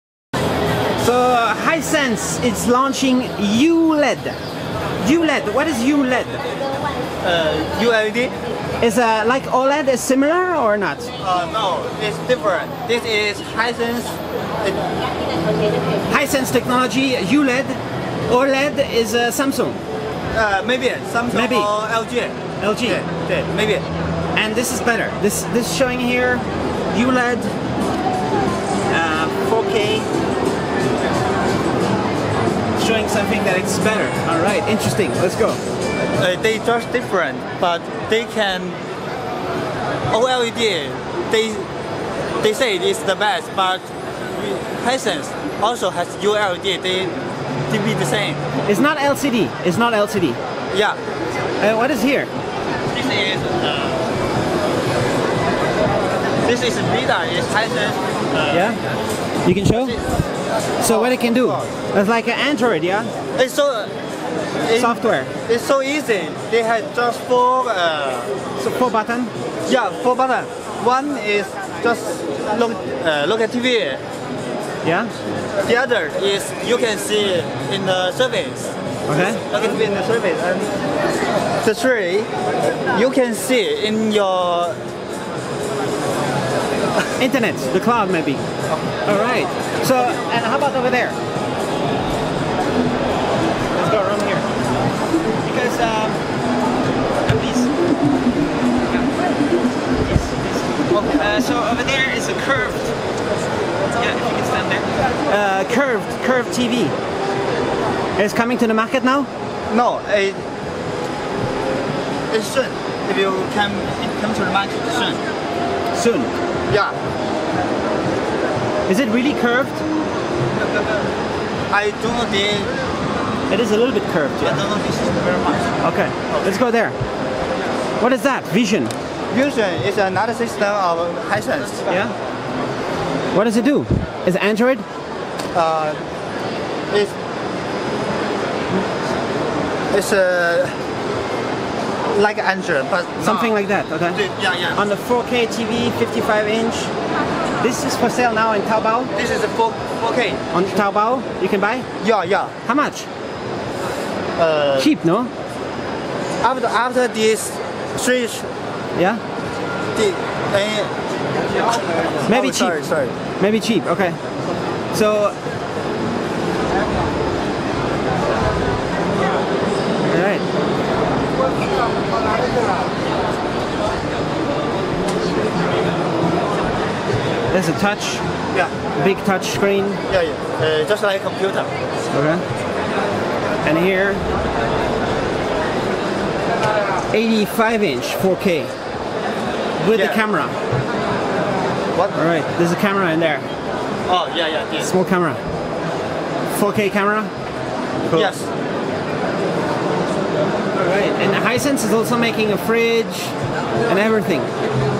Hisense Announcing New ULED Display Technology, 4K TVs, Curved TVs and more
Hisense is a company based in Qingdao China that makes home-used appliances like TVs, refrigerators and other. Here at Shenzhen Electronics Fair they are showing their latest Smart TVs. They are launching their new display technology called ULED. Hisense use their scene engine technology, the chinese company suggest to have the same picture quality/performance like an OLED TV device. They call it ULED and its basically a LCD-LED backlight display with some improvements to match the OLED display technology. Hisense's TVs have their own software and hardware built-in to make the TV smart. The software is some kind of customized operating system similar to android or built over Linux that offers many potentials for home entertainment. Their TVs come in different diagonal sizes and different colors. Some have touchscreen or hand gestures control options and soon a curved TV will be available from Hisense.